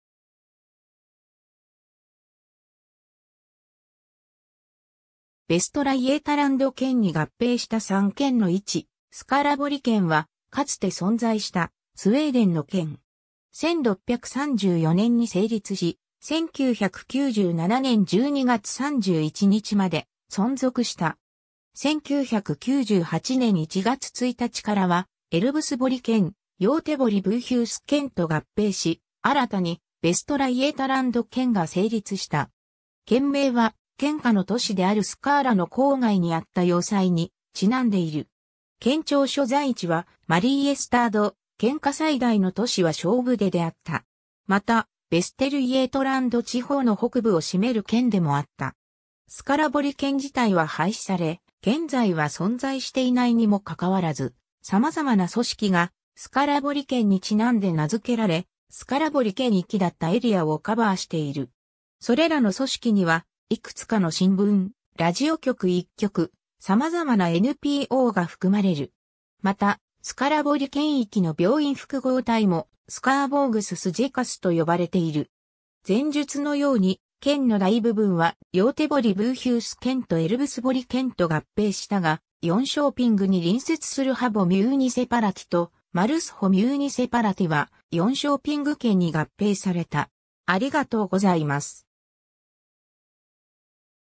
スカラボリ県
スウェーデンの県_(廃止)
ヴェストラ・イェータランド県
ヴェストラ・イェータランド県に合併した3県の位置 (黄色の部分がスカラボリ県域) スカラボリ県 (スカラボリけん、スウェーデン語: Skaraborgs län)は、かつて存在したスウェーデンの県。
1634年に成立し、1997年12月31日まで存続した。
1998年1月1日からはエルヴスボリ県、ヨーテボリ・ブーヒュース県と合併し、新たにヴェストラ・イェータランド県が成立した。
 県名は、県下の都市であるスカーラ (スウェーデン語: Skara)の郊外にあった要塞 (スウェーデン語: borg)にちなんでいる。
県庁所在地はマリーエスタード、県下最大の都市はショーヴデであった。
また、ヴェステルイェートランド地方の北部を占める県でもあった。
 スカラボリ県自体は廃止され、現在は存在していないにもかかわらず、さまざまな組織がスカラボリ県にちなんで名づけられ、スカラボリ県域だったエリアをカバーしている。
それらの組織には、いくつかの新聞、ラジオ局1局(P4 Skaraborg)、様々なNPOが含まれる。
また、スカラボリ県域の病院複合体も、Skaraborgs sjukhusと呼ばれている。
 前述のように、県の大部分はヨーテボリ・ブーヒュース県とエルヴスボリ県と合併したが、ヨンショーピングに隣接するHabo MunicipalityとMullsjö Municipalityは、ヨンショーピング県に合併された。